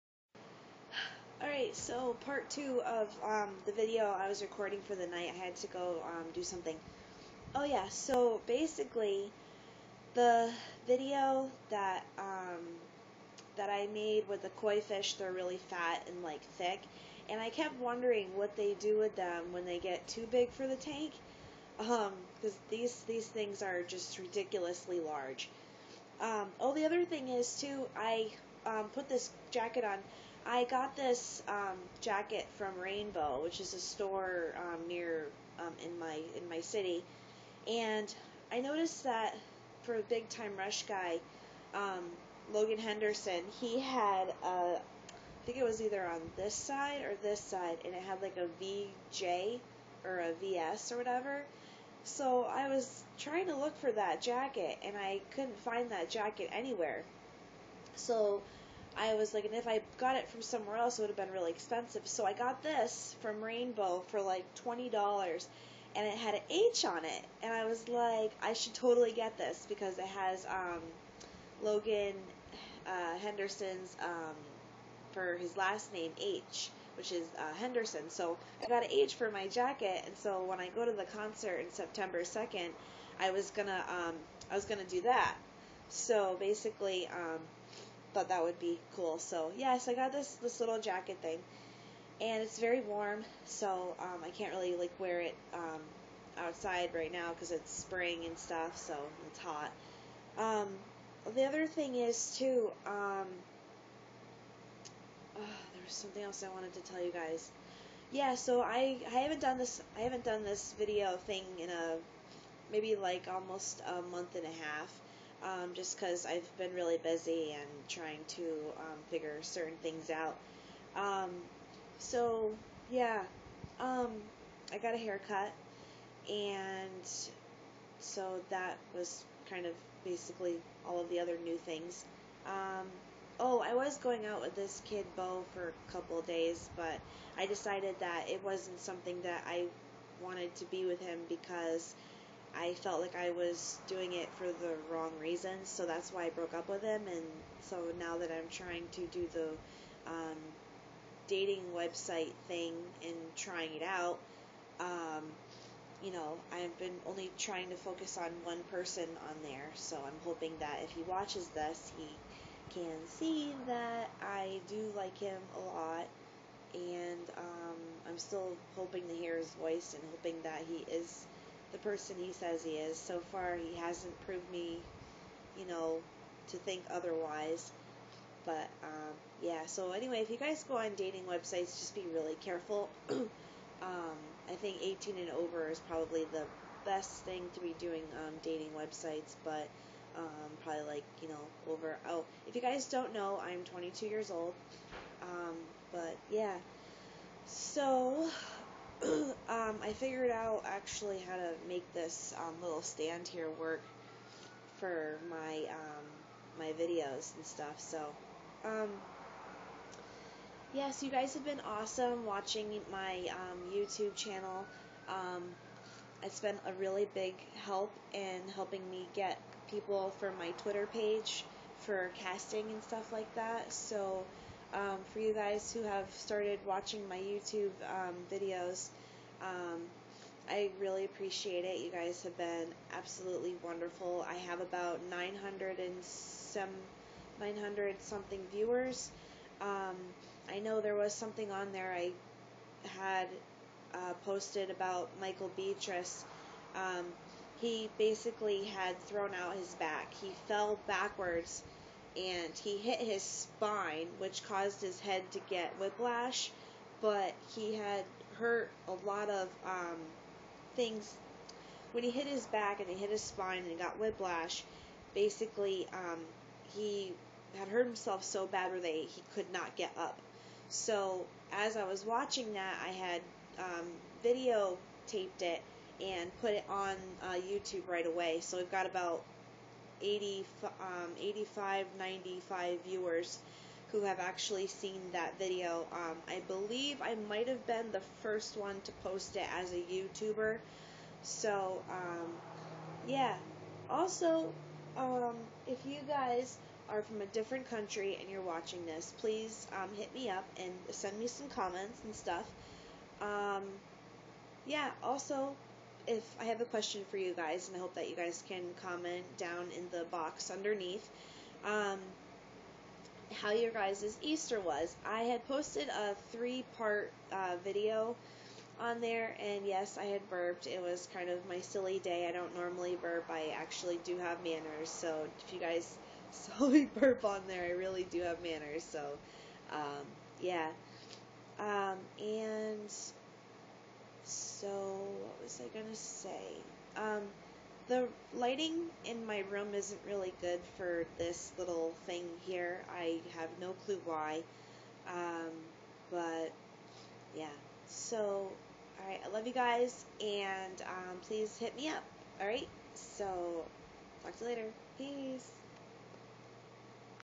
Part 2 of beths weekly update
Plenty of fish online dating prince he's so cute do u think he would still like me.